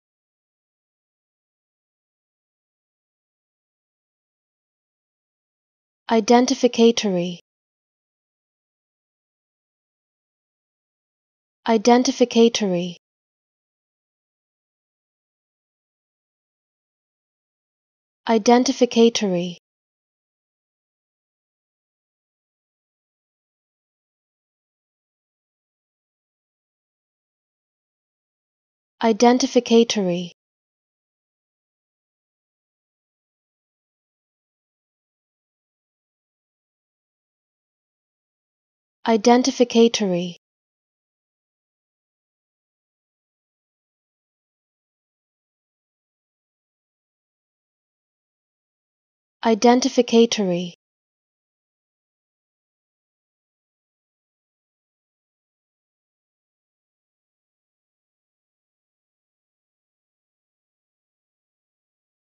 How To Say Identificatory
Pronunciation of Identificatory: Learn how to pronounce the word Identificatory.